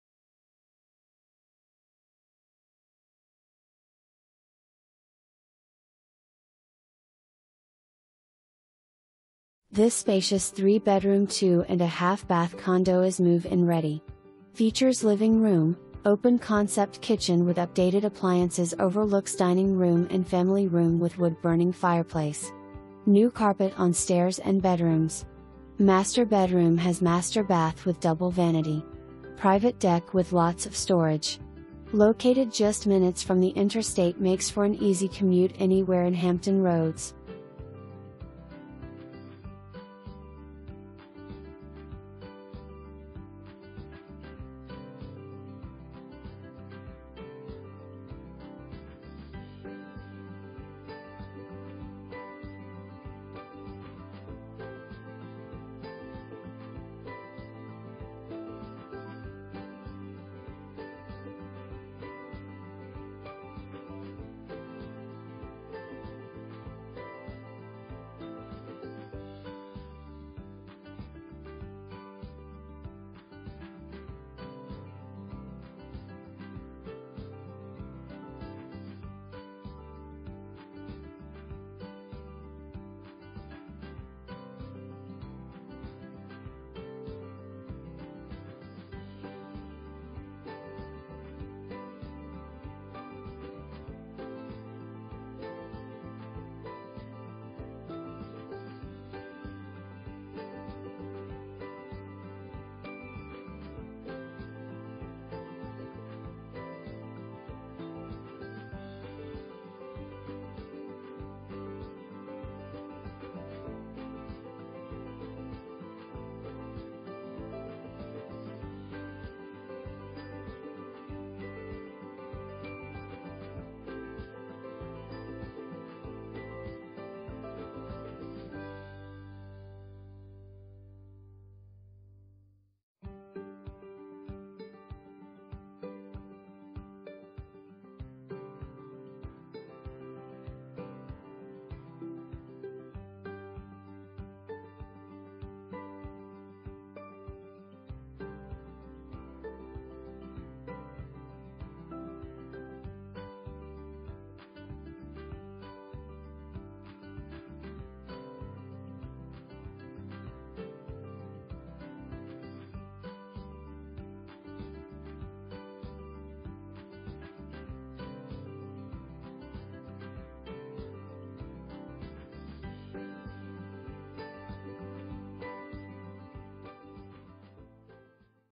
3606 Cinnamon Court
This spacious 3 bedroom 2 ½ bath condo is move-in-ready. Features living room, open concept kitchen with updated appliances overlooks dining room and family room with wood burning fireplace. New carpet on stairs and bedrooms. Master bedroom has master bath with double vanity . Private deck with lots of storage. Located just minutes from the interstate makes for an easy commute any where in Hampton Roads.

2 full bath, 1 half bath

David Wilkey
Rose & Womble Realty Company